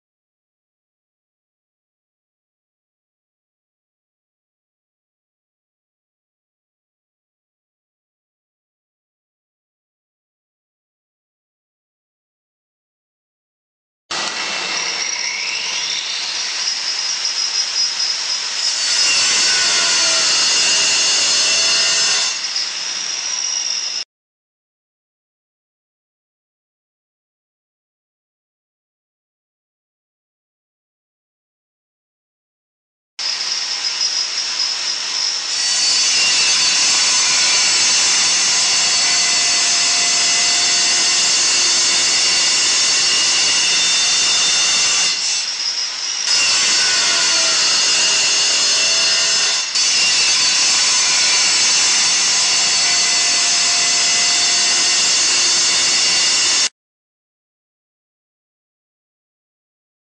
Test tarczy diamentowej Stayer Cerámica Banda Contínua
Tarcze diamentowe Stayer Cerámica Banda Contínua to narzędzia do cięcia średnio i mało twardej ceramiki budowlanej (płytki ścienne i podłogowe, dachówki itp.), kamienia i bruku. W redakcyjnym warsztacie bliżej przyjrzeliśmy się możliwością roboczym tych tarcz...